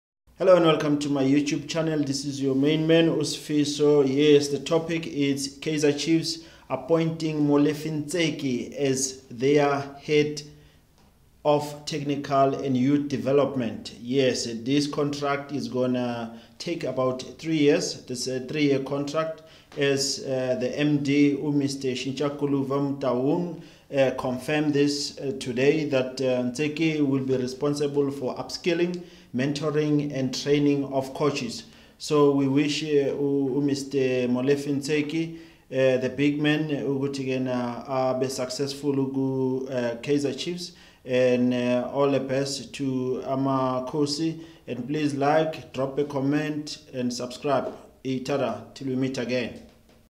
Kaizer Chiefs fc Appoints Molefi Ntseki as Head of Technical Department
Kaizer Chiefs fc Appoints Molefi Ntseki as their Head of Technical and youth development.
The contract is for 3 years. Arthur Zwane and Dillon Sheppard are still in charge in their positions.